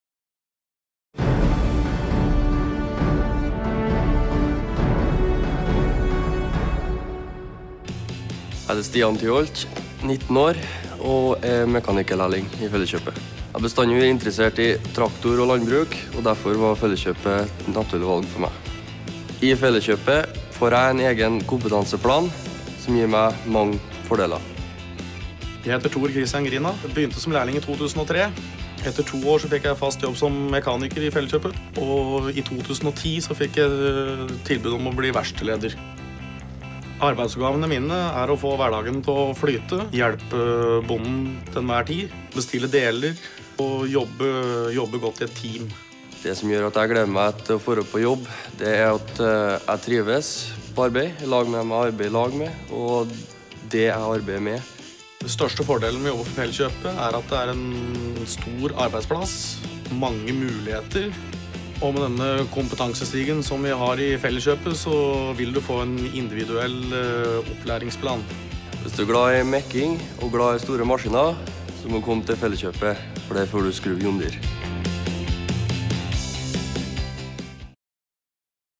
Karriere i Felleskjøpet Agri -- VERKSTED
Denne filmen viser hvordan det er å jobbe som mekaniker og verkstedleder i Felleskjøpet Agri!